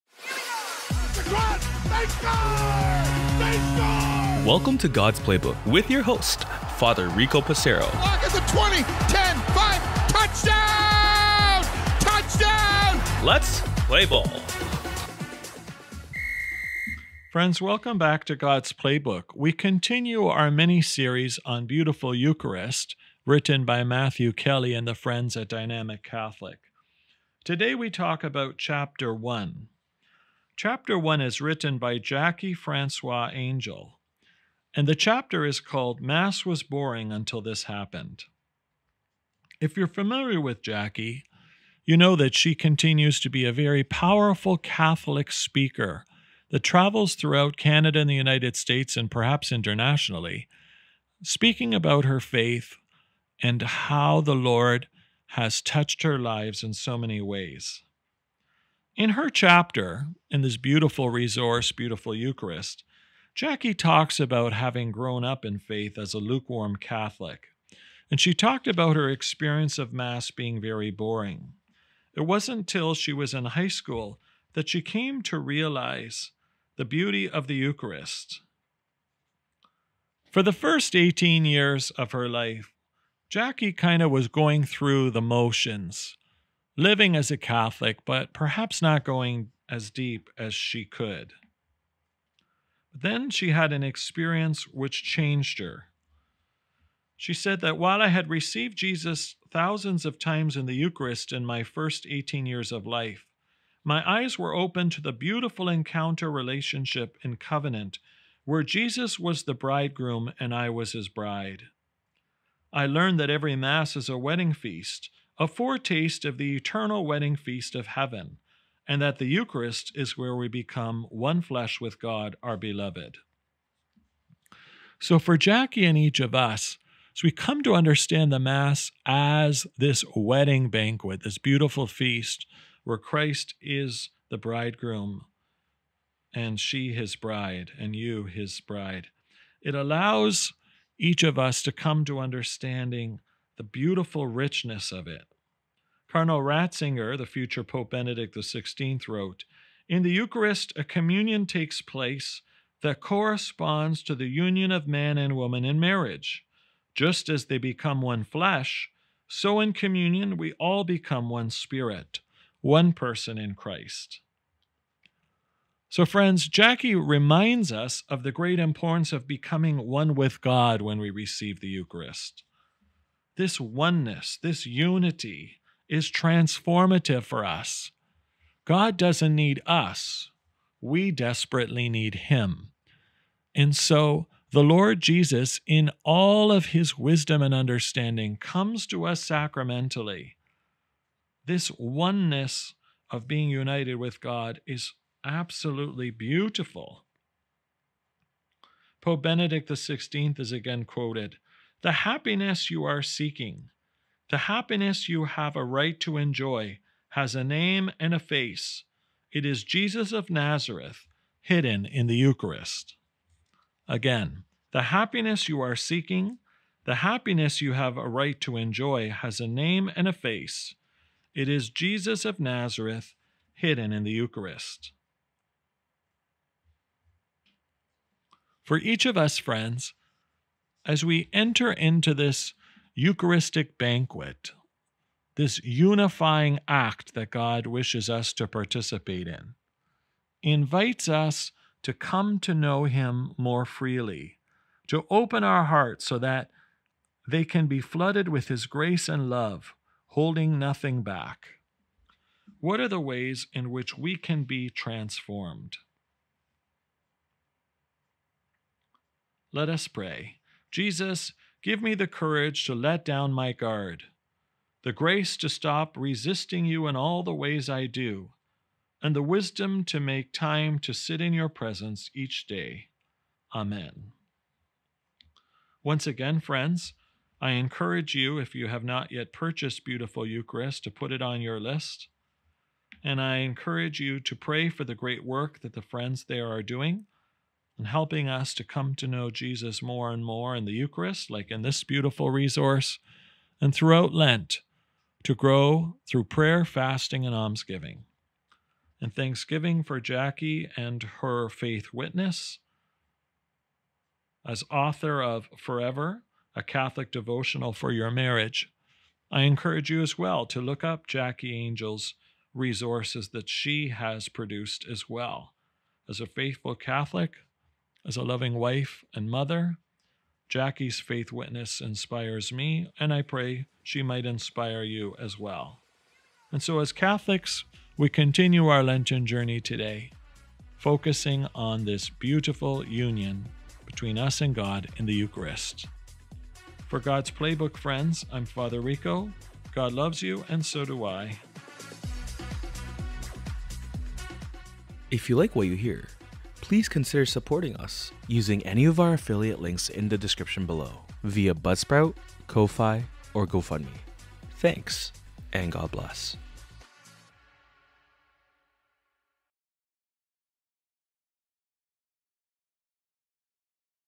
God's Playbook - S4 Episode 63 - Lenten Mission Series - Beautiful Eucharist - Chapter 1
Are you a lukewarm catholic? Do you find mass boring?  Are you just going through the motions of day-to-day life, not attempting to deepen your faith? Let's discuss and talk about a certain individual who had an experience which changed her life. Let's learn more on today's podcast episode alongside Fr. Rico.